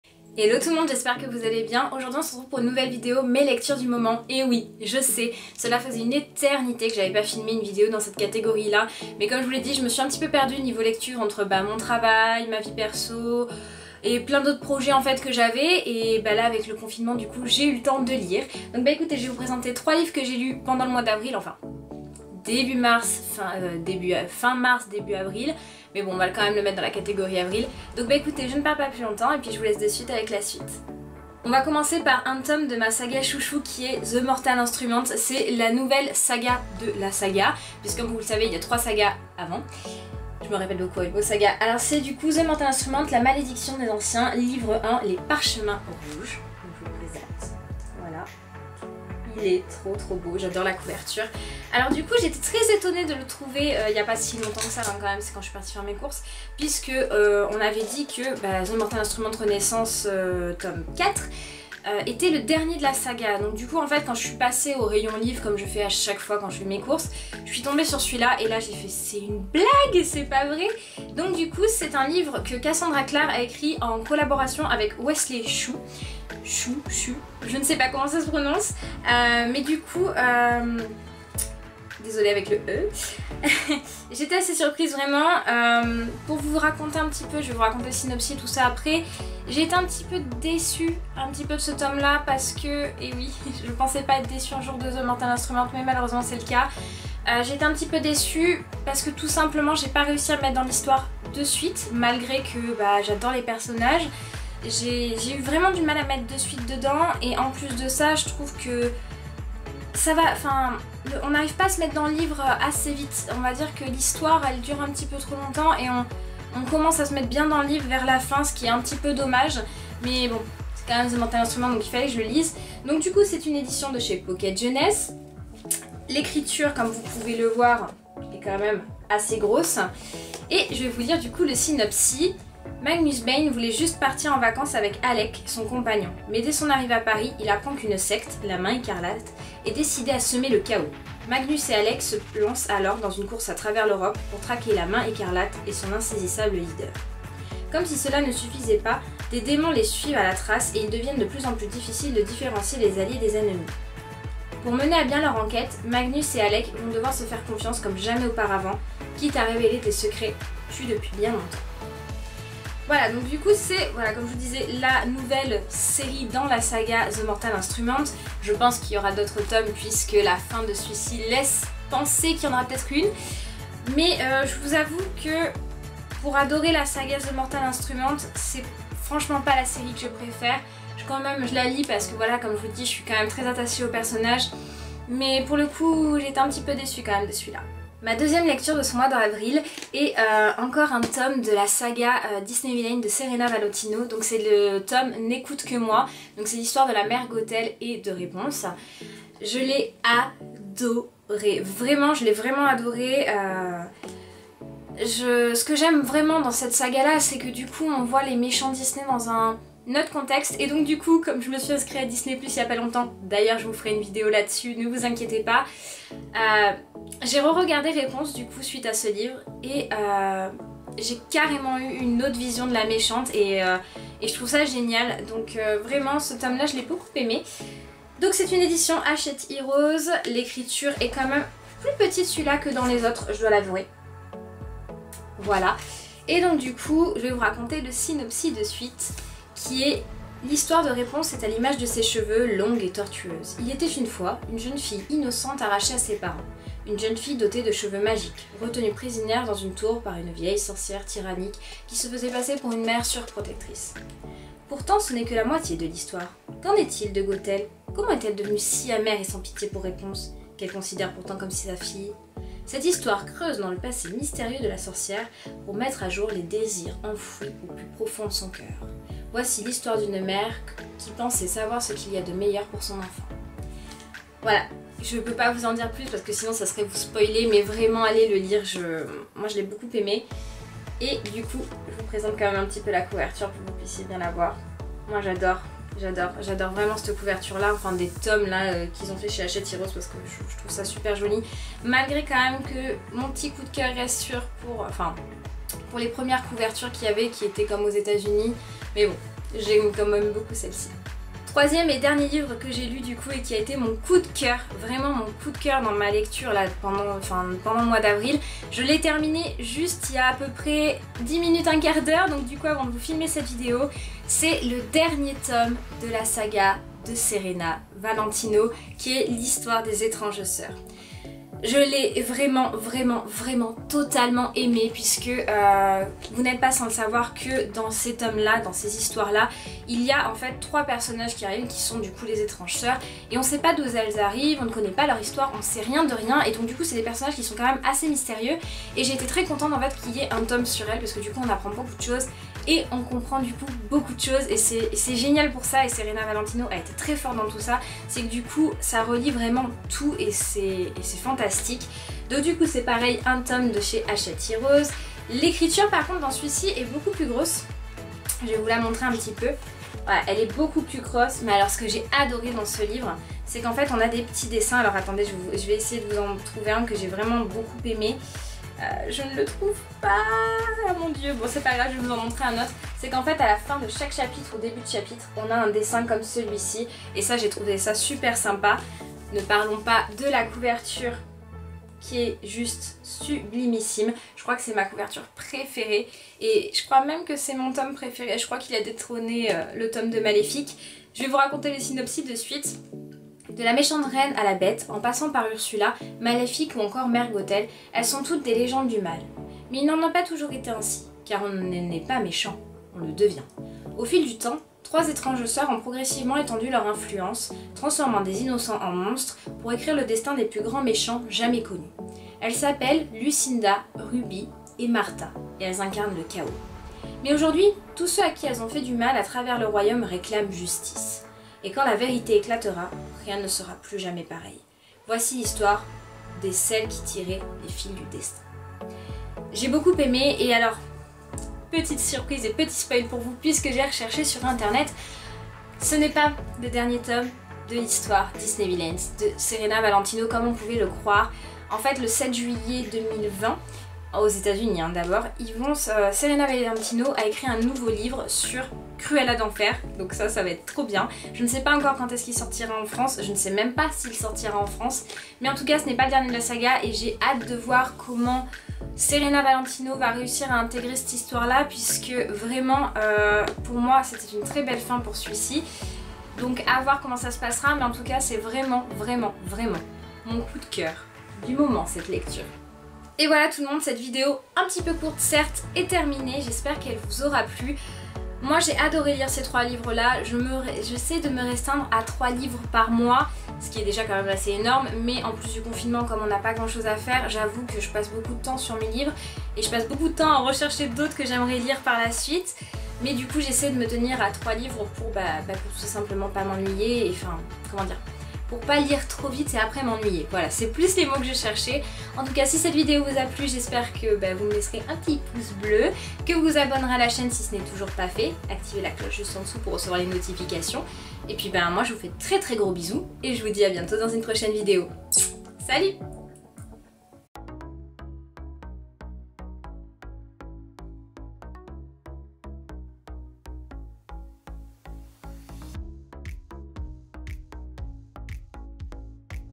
Mes Lectures Du Moment - Avril 2020
Ça y est, la catégorie "mes lectures du moments" reprend du service. Après plusieurs mois sans vidéos avec pour thème les livres que j'ai pu lire, en voici une nouvelle, j'espère qu'elle vous plaira !

Ce que je porte :

Haut : Bershka
Collier : My jolie candle
Rouge à lèvre : "Mull it over" de chez Mac Cosmetics

LIVRES MENTIONNÉS DANS LA VIDÉO :

- "The mortal instruments : la malédiction des anciens" Tome 1 de Cassandra Clare et Wesley Chu

Cheyenne ♡